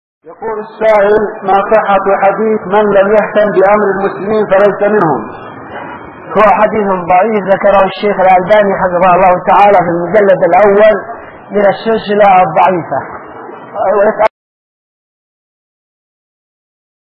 حديث ضعيف : من لم يهتم بأمر المسلمين فليس منهم
قال الشيخ مقبل الوادعي : " من لم يهتم بأمر المسلمين فليس منهم " هو حديث ضعيف ، ذكره الشيخ الألباني في المجلد الأول من السلسلة الضعيفية